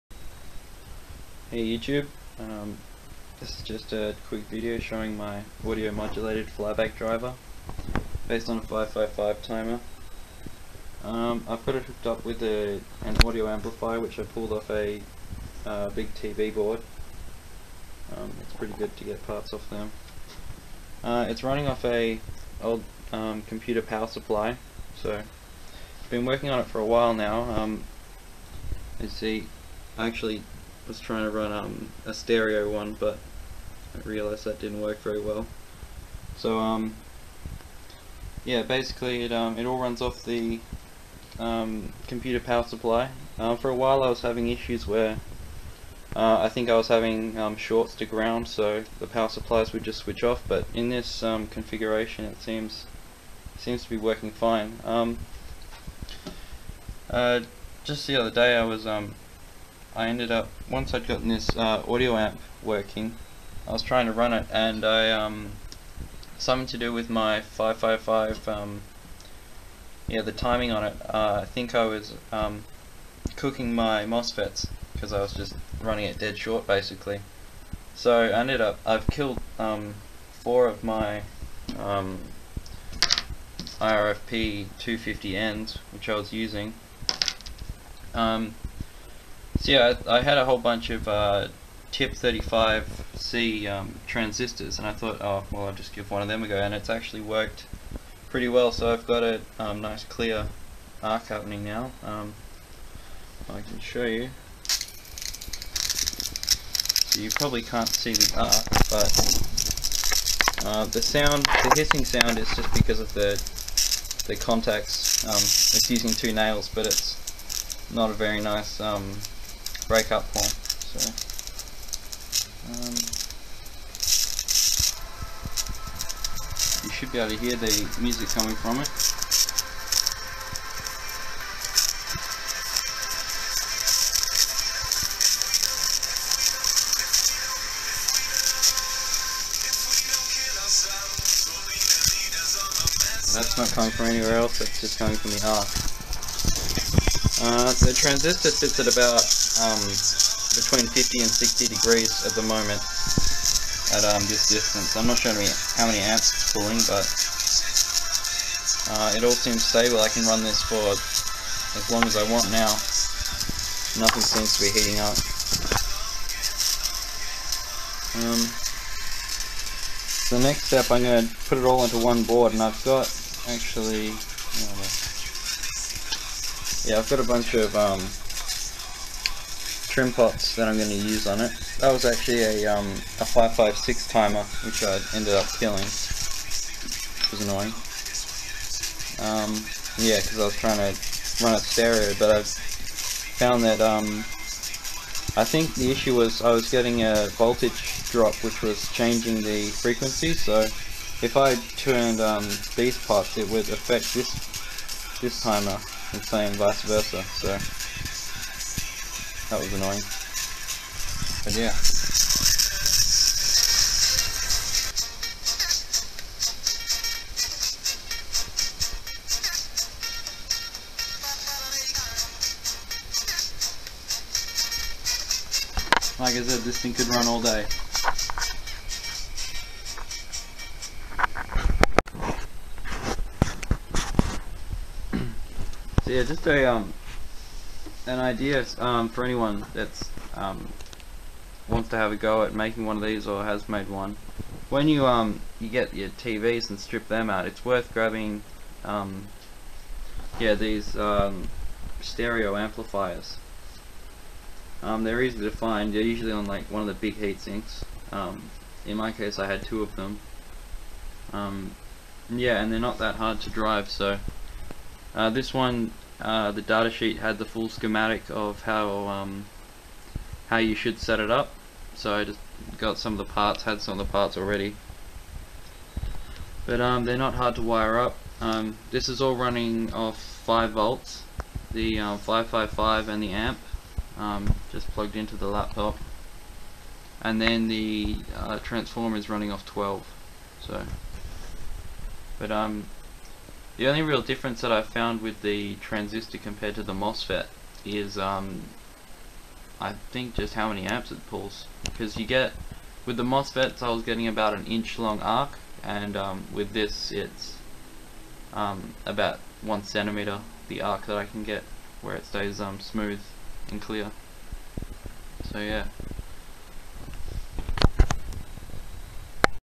555 timer based plasma speaker
This is just video to share my audio modulated flyback transformer setup.
Its based around a simple 555 timer in monostable configuration.
The audio amplifier is a 25W TDA7265 stereo amplifier chip that i pulled off a TV board
Im using a TIP35c power transistor instead of a mosfet because i fried all the irfp250n mosfets that i had, im not sure why but they didnt seem to like the driver at all and just got stupid hot.